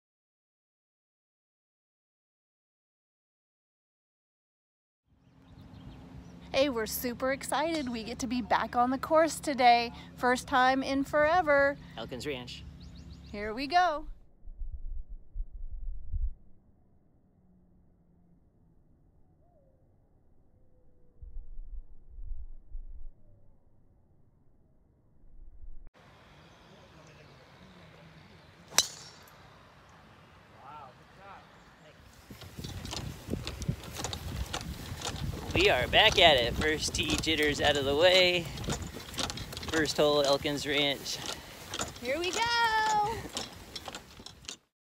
Elkins Ranch GC
First time out since the stay at home orders. First tee jitters, not really.